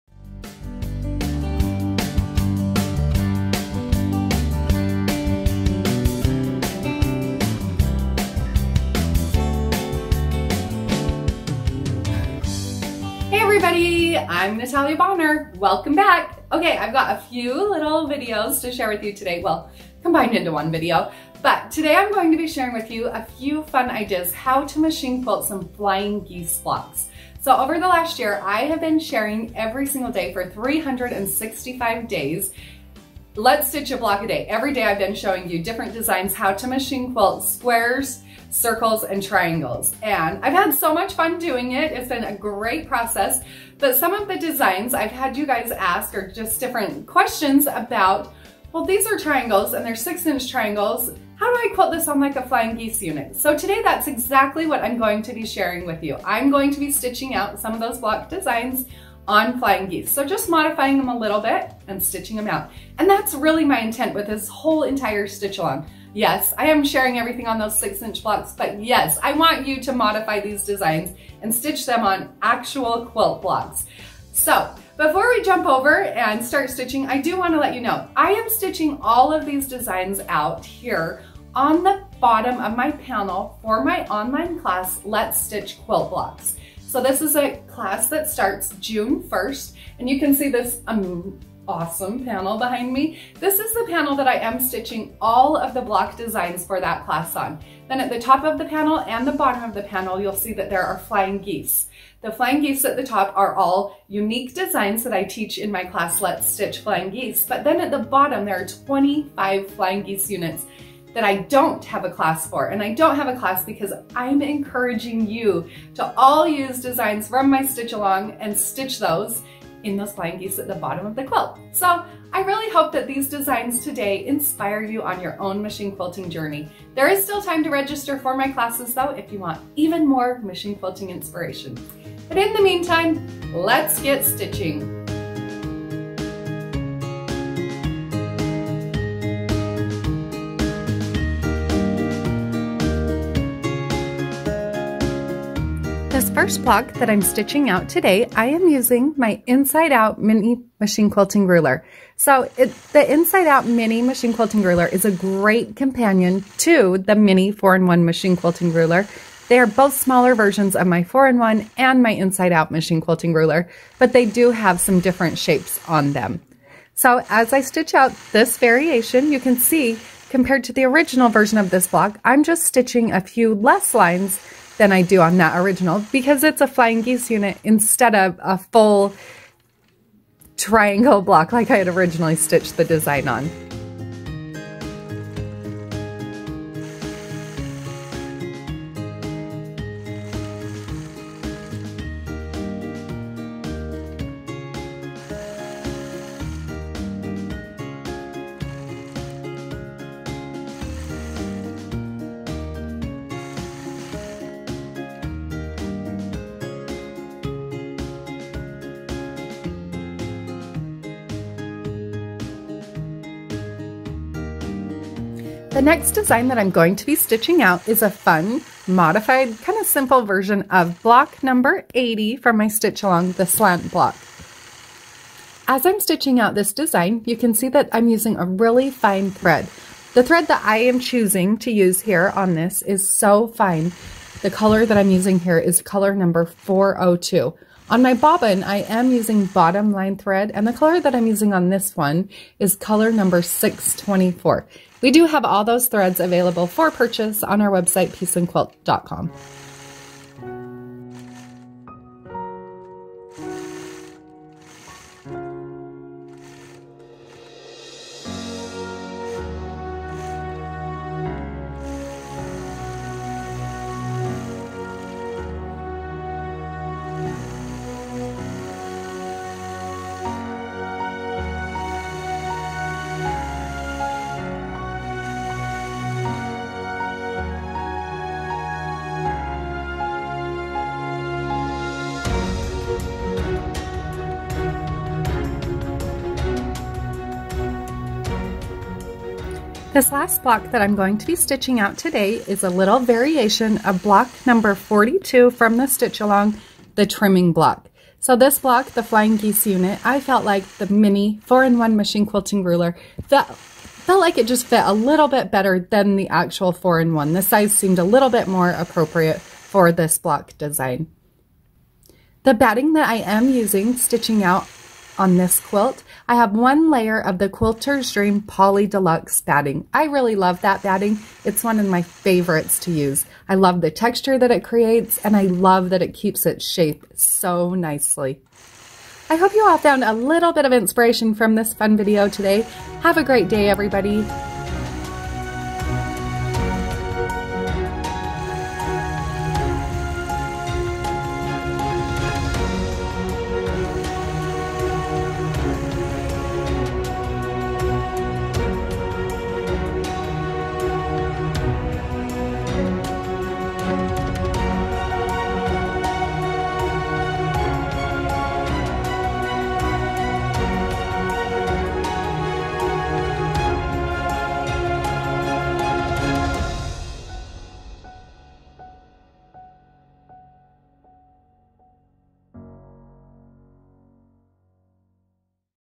3 Ways to Quickly Quilt Flying Geese Quilt Blocks with Natalia Bonner
For more details about the Stitch-Along and the products Natalia is using, visit her blog